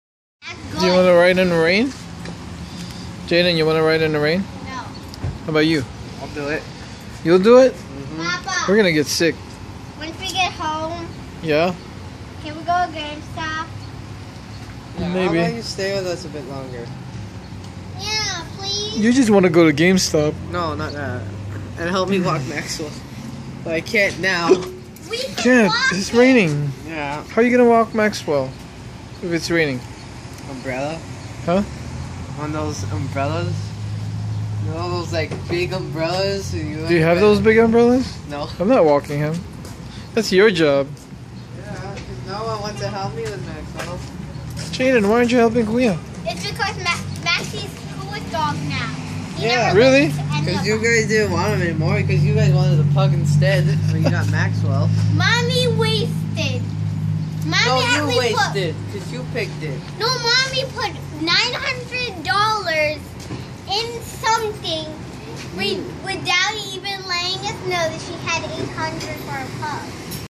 Brandon and Jayden went riding with me.
I love my kids so much.  I  wish I could be with them all the time.